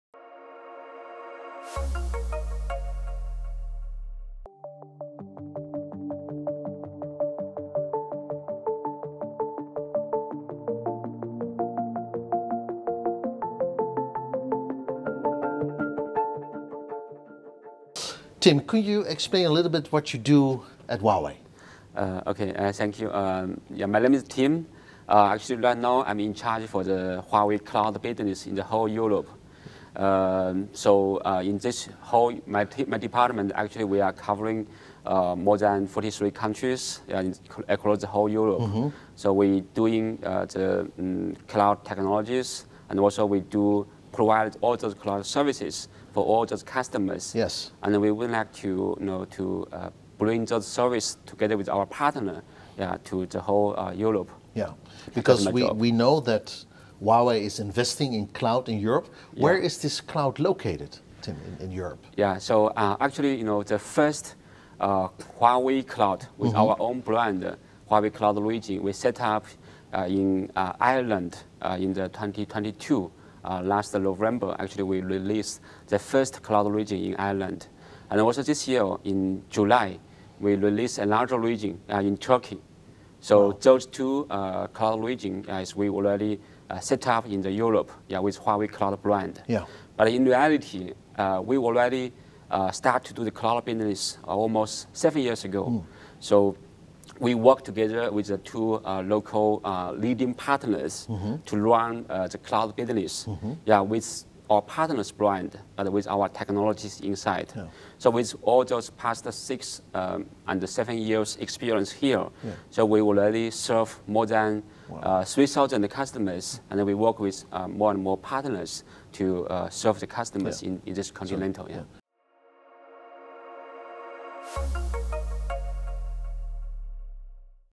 Danny Frietman spreekt Tim Tao over Huawei Cloud
Huawei Cloud is van plan zijn positie als vertrouwde partner voor bedrijven en organisaties in diverse sectoren te verstevigen. Dat doet Huawei Cloud onder andere vanuit zijn cloud datacenters in Ierland en Turkije. Danny Frietman, moderator bij Dutch IT Channel en Dutch IT Leaders, sprak hierover in het Huawei Technologies kantoor in Rijswjk met Tim Tao, President van de Huawei Cloud West Europe Region.

Huawei bedient samen met zijn partners met cloud diensten diverse topspelers in verschillende sectoren, waaronder financiële instellingen, de automobielsector, media, e-commerce en gaming, bediend met baanbrekende Huawei Cloud-oplossingen zo meldt Van Katwijk.

Het Huawei Cloud Partner Network (HCPN) ondersteunt partners daarin zoals met GoCloud-programma’s, bij het opbouwen van aanbod, het ontwikkelen van competenties en het creëren van toegevoegde waarde.

Huawei Cloud biedt ook een Software Partner-programma, gericht op het ondersteunen van de strategie van Huawei Cloud op het gebied van cloudinfrastructuren en PaaS-diensten. Huawei biedt in 2024 stimuleringsprogramma's aan, waaronder het Partner Customer Engagement-programma, het Migration Acceleration-programma, het MSP Cloud Transformation Support-programma en het Enterprise Discount-programma.

Verder biedt Huawei Cloud ondersteuning vanuit het European Partner GTM Centre, dat Go-To-Market (GTM) ondersteuning zal bieden met materialen en campagnes.